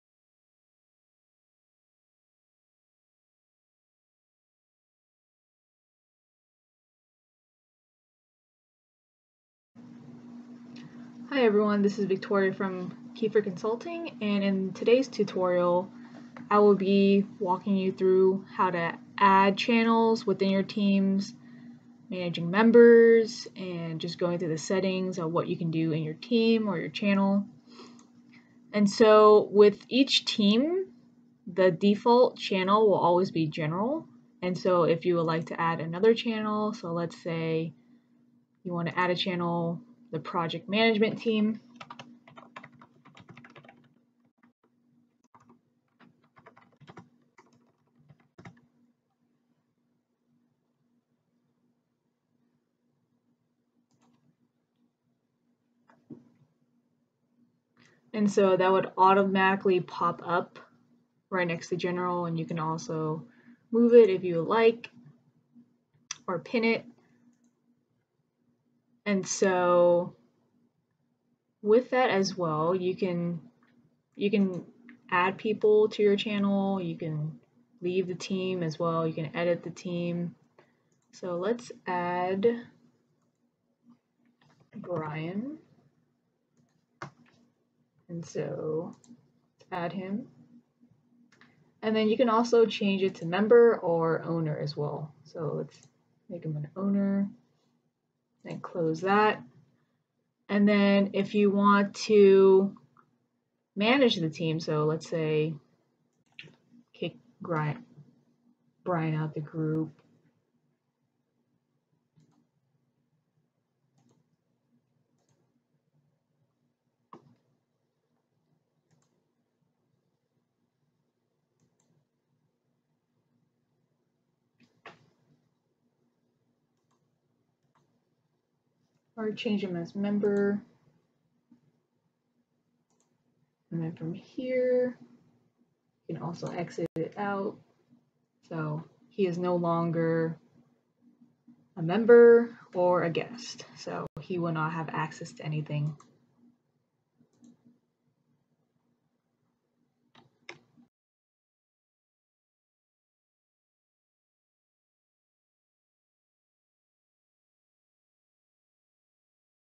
Creating a Microsoft Teams Channel and Adding or Removing Members
Video will show you how to create a Microsoft Teams Channel and add or removing members.